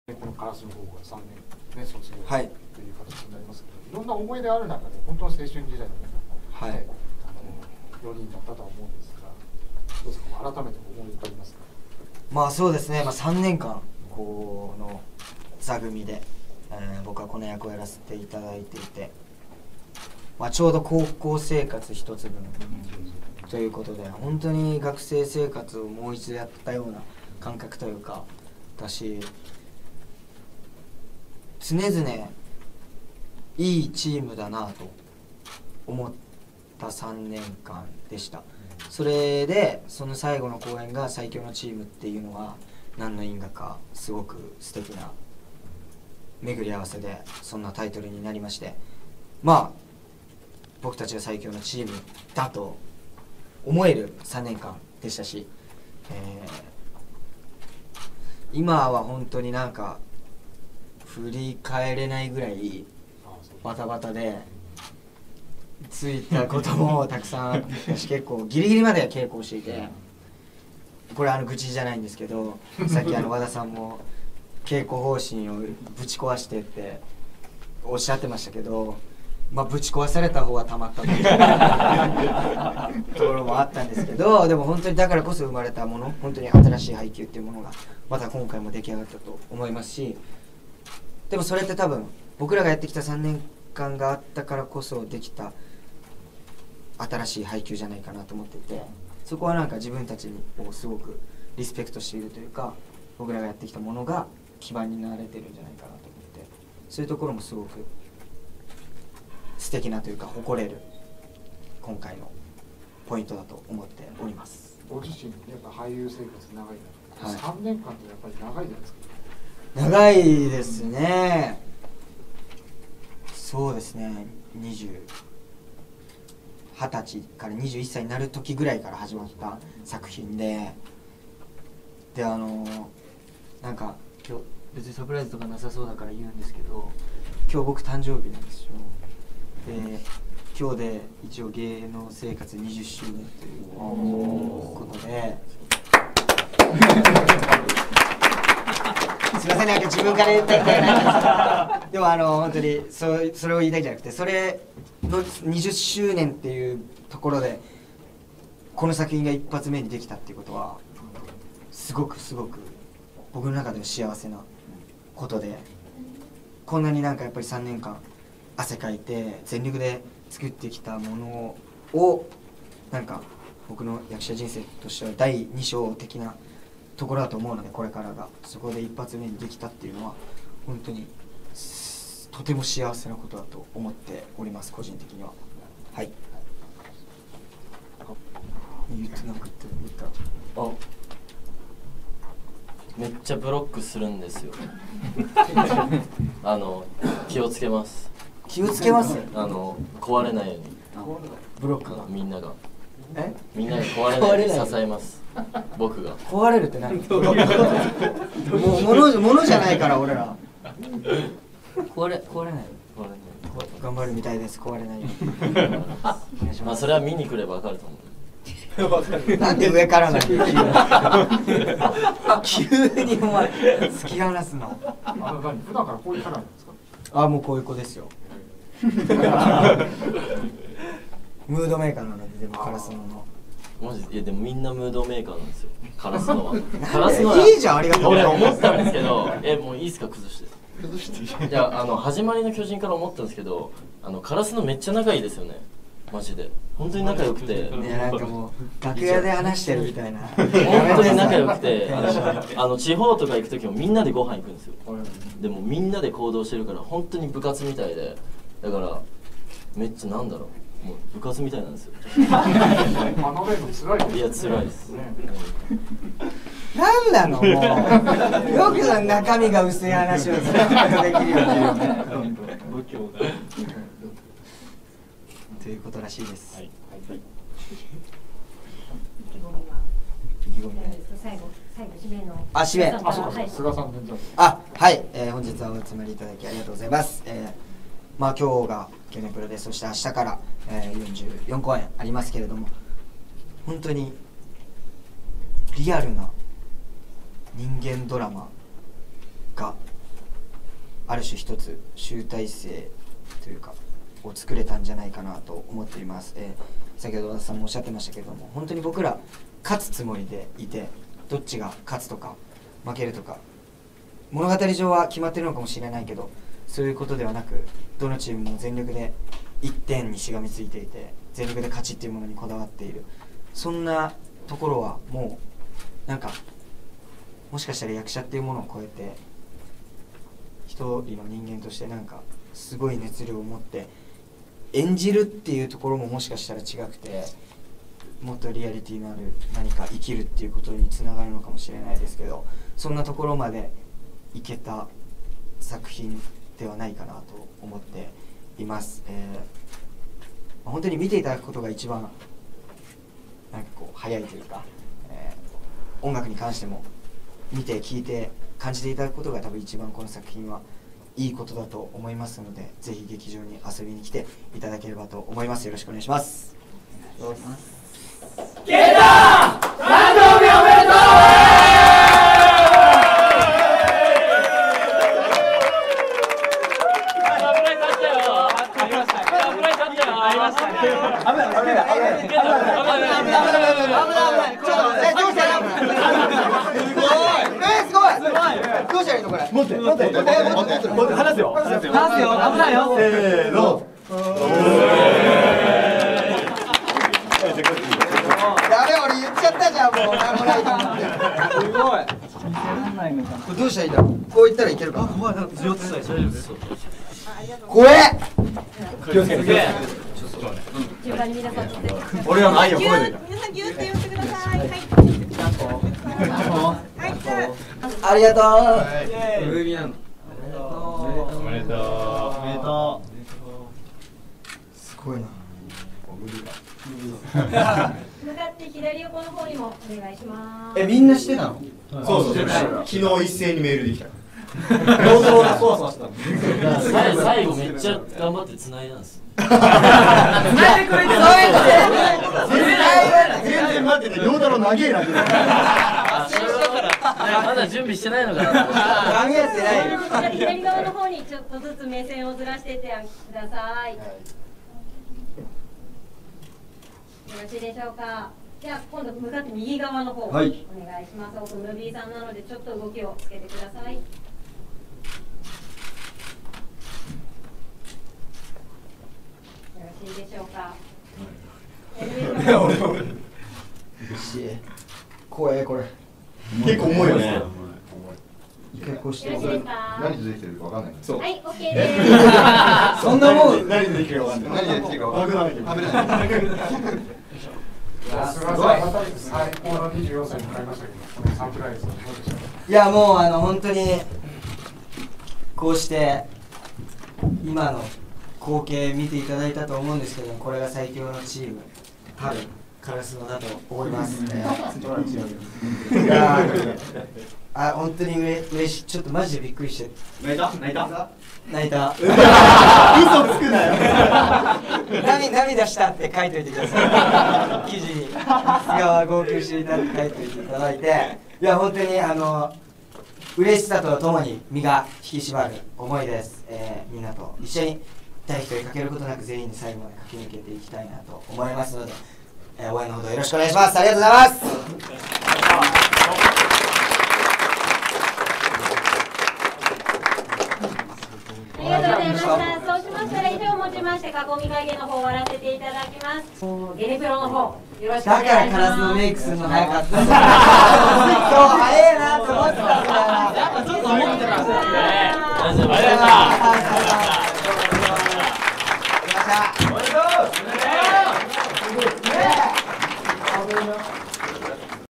【会見2/2】ﾊｲﾊﾟｰﾌﾟﾛｼﾞｪｸｼｮﾝ演劇「ハイキュ―!!」〝最強の場所（チーム）""初日前会見・質疑応答・バースデーサプライズ編
2018年10月19日に行われた初日前会見の後半部分で、質疑応答と須賀健太へのバースデーサプライズを掲載。
前編は、キャスト・スタッフが意気込みを語ります。
【会見出席者】
須賀健太、影山達也、小坂涼太郎、三浦海里、遊馬晃祐、有田賢史、ウォーリー木下（演出・脚本】、和田俊輔（音楽・演奏）
塩田康平、渕野右登、川原一馬、田中啓太、田中尚輝、冨森ジャスティン

【公演情報】
ﾊｲﾊﾟｰﾌﾟﾛｼﾞｪｸｼｮﾝ演劇「ハイキュ―!!」〝最強の場所（チーム）""
●東京公演　2018年10月20日～28日＠TOKYO DOME CITY HALL
●広島公演　2018年11月9日～10日＠はつかいち文化ホール　さくらぴあ　大ホール
●兵庫公演　2018年11月15日～18日＠あましんアルカイックﾎｰﾙ
●大阪公演　2018年11月23日～25日＠梅田芸術劇場　メインホール
●宮城公演　2018年11月30日～12月2日＠多賀城市民会館　大ホール
●東京公演　2018年12月7日～16日＠日本青年館ホール